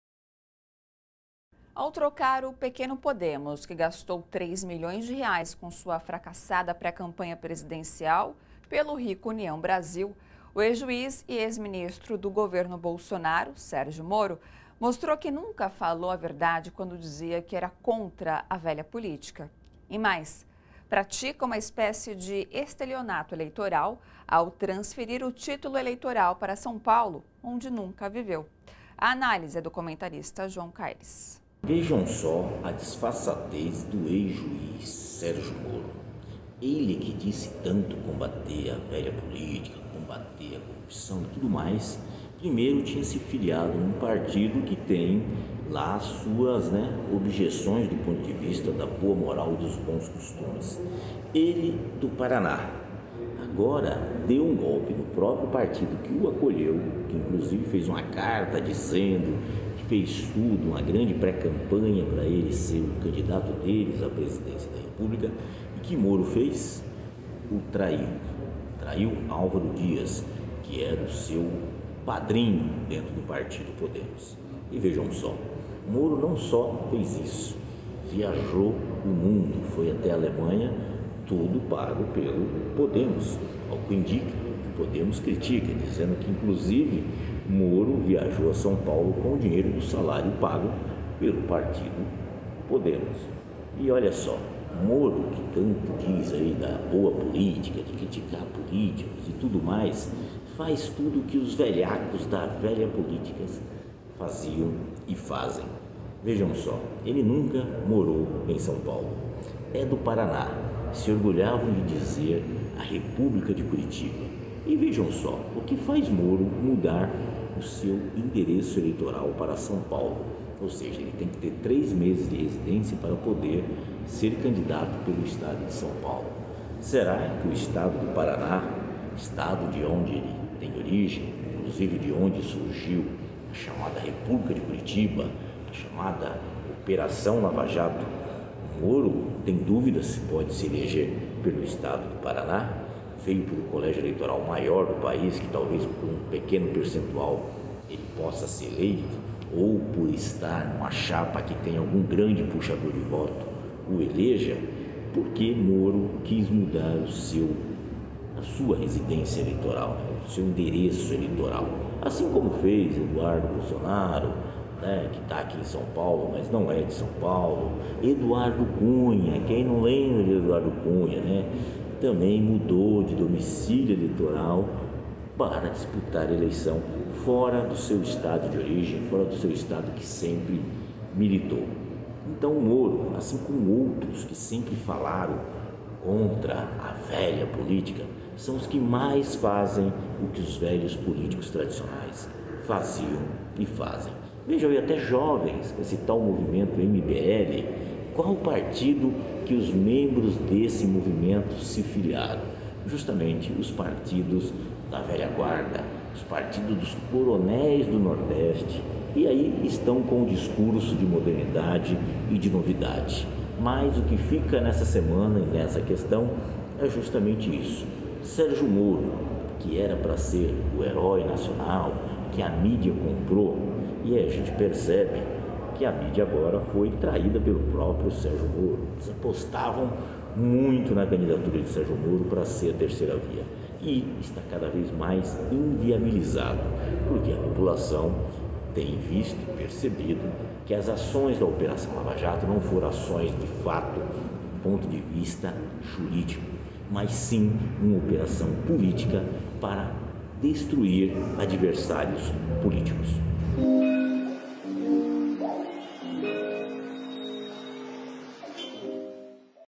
Depois de trair a Justiça, agora Sergio Moro trai o Podemos
Ao trocar o pequeno Podemos, que gastou R$ 3 milhões com sua fracassada pré-campanha presidencial, pelo rico União Brasil, Sergio Moro mostrou que nunca falou a verdade quando dizia que era contra a velha política.